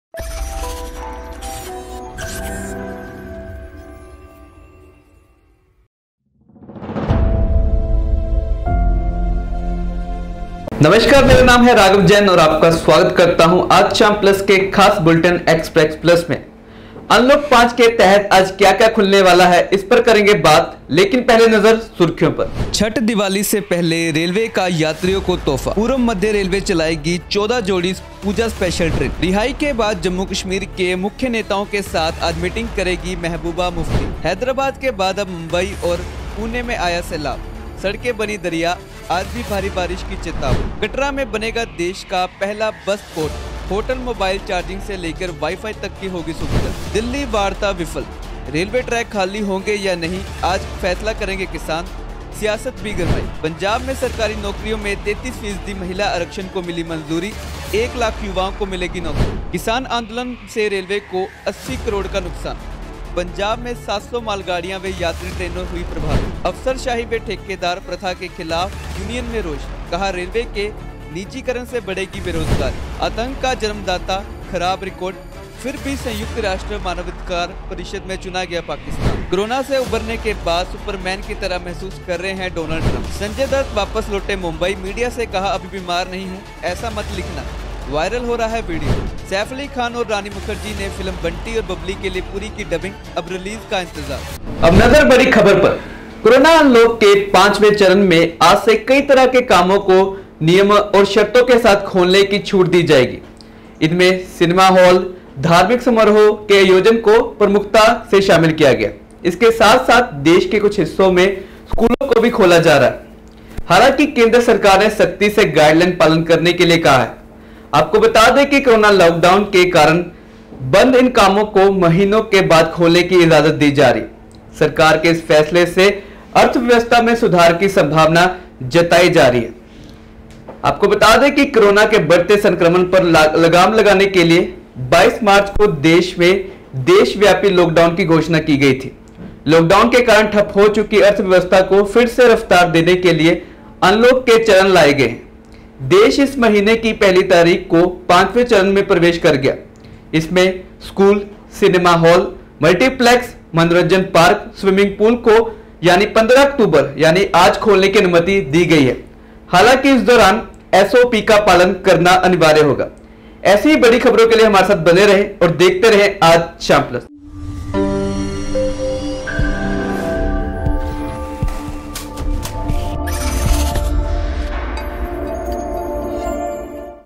Express plus |Unlock 5.0: स्कूल और सिनेमा हॉल के अलावा आज से खुल रहा है और भी बहुत कुछ | Raghav jain
🖋Unlock 5.0: स्कूल और सिनेमा हॉल के अलावा आज से खुल रहा है और भी बहुत कुछ

🖋आज के Express Plus में हम बात करेंगे कि अनलॉक 5.0 school और Cinema hall के अलावा आज क्या-क्या खुल रहा है। रेल रोको आंदोलन पर आज होगा फैसला। कोरोना के बाद क्या कहा Trump ने इस पर भी होगी नजर। बॉलीवुड खबरें बात होगी सुशांत की बहन की।